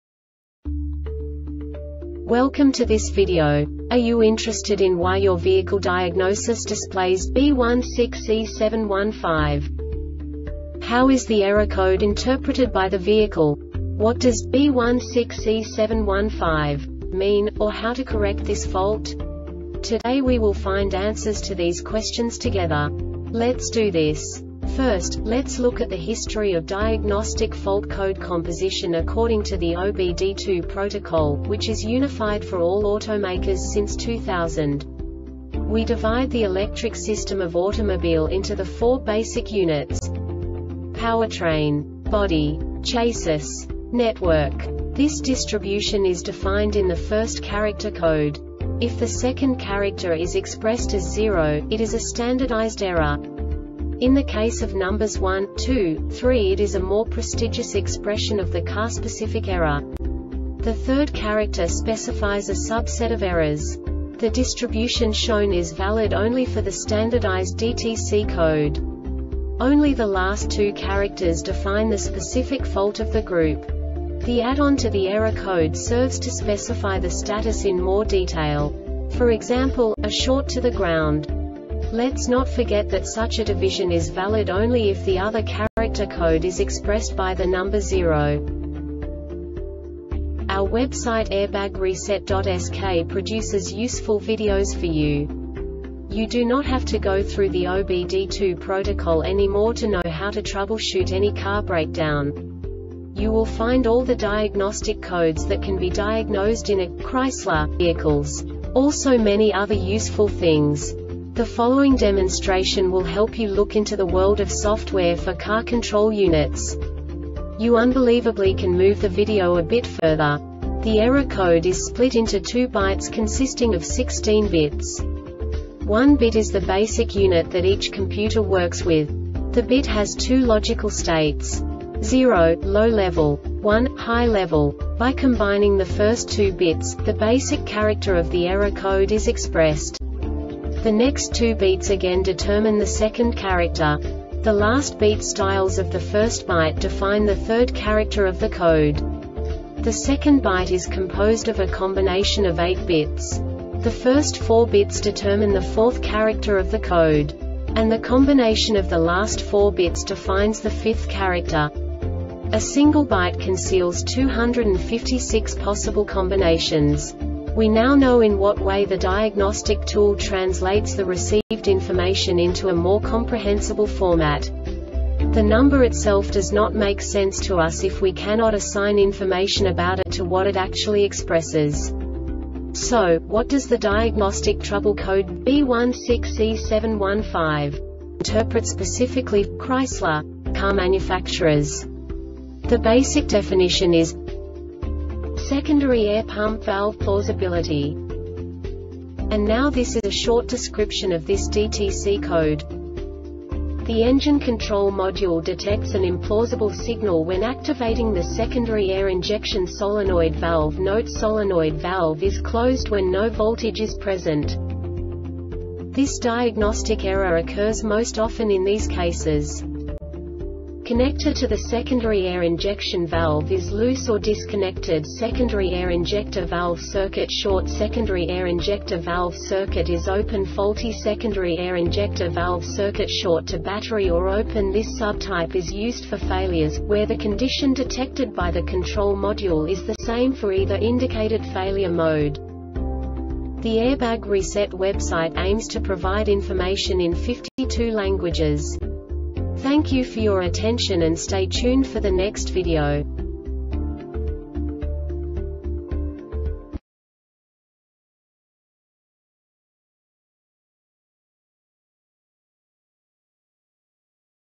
DTC Chrysler B16E7-15 Short Explanation
Contents:
0:21 Basic DTC analysis according to OBD2 protocol standard.
1:48 Insight into programming
2:58 A diagnostically understandable description of the Diagnostic Trouble Code
3:05 Basic error definition

Make:
Chrysler
Code:
B16E7 15
Definition:
Secondary Air Pump Valve Plausibility
Description:
The Engine Control Module detects an implausible signal when activating the secondary air injection solenoid valve. Note: Solenoid valve is closed when no voltage is present.
Cause:
Connector to the secondary air injection valve is loose or disconnectedSecondary air injector valve circuit shortSecondary air injector valve circuit is openFaulty secondary air injector valve
Failure Type: